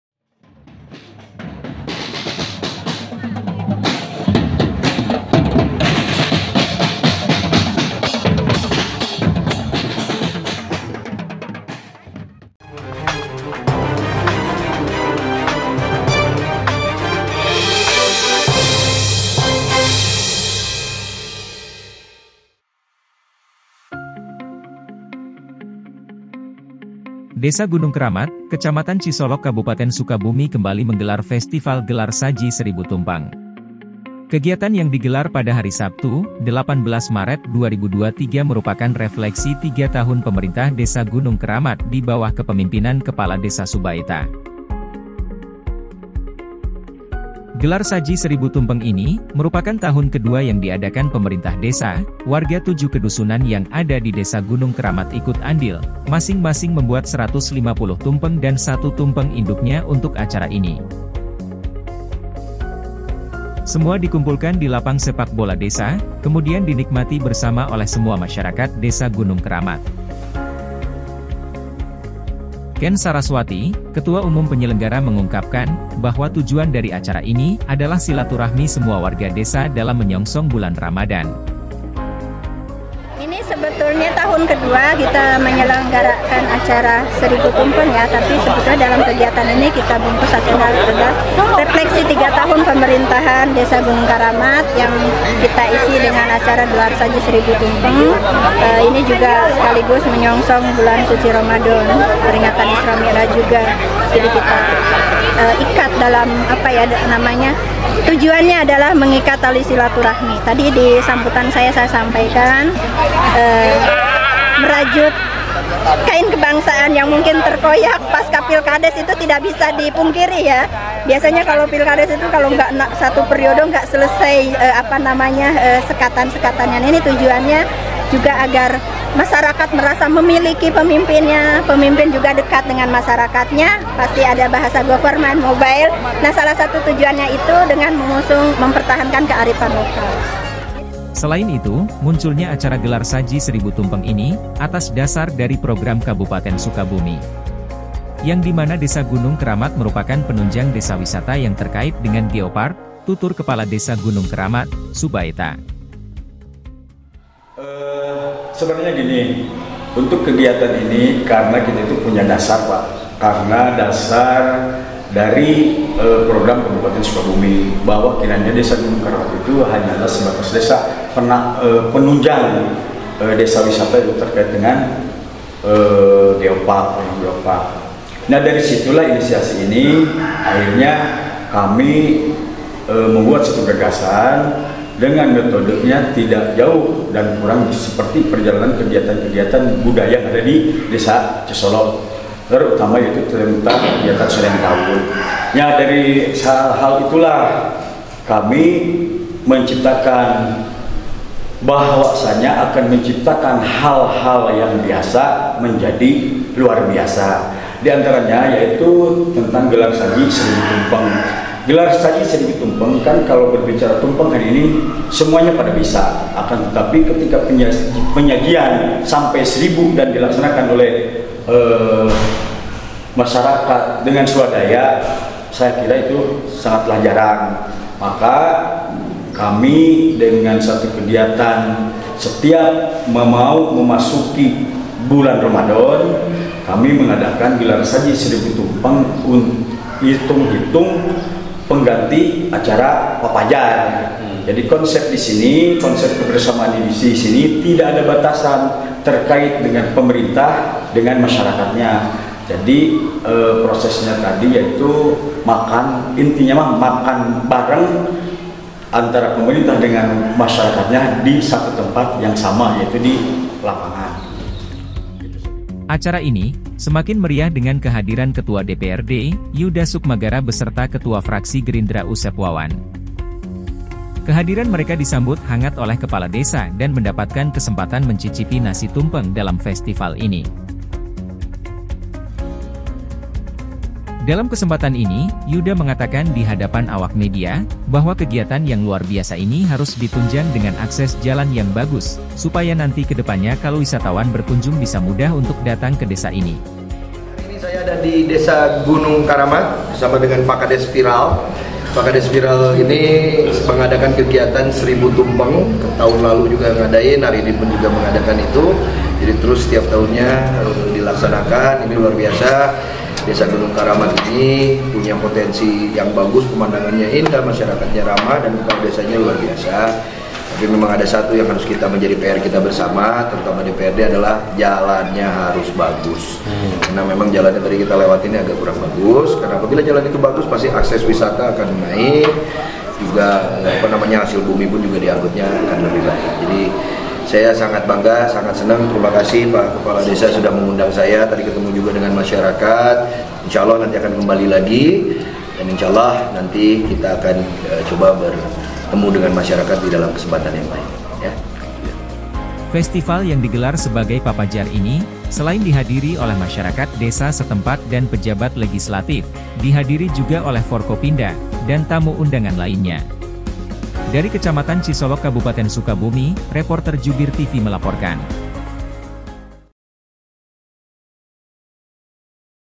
1000 TUMPENG SEBAGAI PENGIKAT SILATURAHMI MASYARAKAT DESA GUNUNG KERAMAT KAB. SUKABUMI
Kab. Sukabumi, Desa Gunung Keramat kec. Cisolok Kabupaten Sukabumi  kembali menggelar FESTIVAL GELAR SAJI 1000 TUMPENG.

Kegiatan yang di gelar pada hari Sabtu, 18 Maret 2023 merupakan refleksi 3 tahun pemerintah desa gunung keramat di bawah kepemimpinan kepala desa subaeta.

Gelar saji 1000 tumpeng ini, merupakan tahun kedua yang diadakan pemerintah desa, warga 7 kedusunan yang ada di Desa gunung keramat ikut andil, masing masing membuat 150 tumpeng dan 1 tumpeng induknya untuk acara ini

Semua di kumpulkan di lapang sepak bola desa, kemudian dinikmati bersama oleh semua masyarakat desa gunung keramat.

KEN SARASWATI, Ketua Umum Penyelenggara mengungkapkan, bahwa tujuan dari acara ini adalah silaturahmi semua warga Desa dalam menyongsong bulan Ramadhan.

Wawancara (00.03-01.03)

Selain itu, Munculnya Acara Gelar saji 1000 tumpeng ini, atas dasar dari program Kabupaten Sukabumi. Yang di mana desa gunung keramat merupakan penunjang  desa wisata yang terkait dengan geopark, tutur Kepala Desa gunung keramat, SUBAETA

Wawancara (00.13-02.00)

Acara ini, semakin meriah dengan kehadiran Ketua DPRD, YUDHA SUKMAGARA beserta Ketua Fraksi Gerindra Usep wawan.

Kehadiran mereka disambut hangat oleh kepala desa dan mendapatkan kesempatan mencicipi Nasi tumpeng dalam festival ini.

Dalam kesempatan ini, Yudha mengatakan di hadapan awak media, bahwa kegiatan yang luar biasa ini harus di tunjang dengan akses jalan yang bagus, supaya nanti kedepannya kalo wisatawan berkunjung bisa mudah untuk dating ke desa ini.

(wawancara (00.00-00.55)

Festival yang digelar sebagai papajar ini, selain dihadiri oleh masyarakat desa setempat dan pejabat legislative, dihadiri juga oleh forkominda, dan tamu undangan lainnya.

Dari Kecamatan Cisolok Kabupaten sukabumi, Reporter Jubir TV melaporkan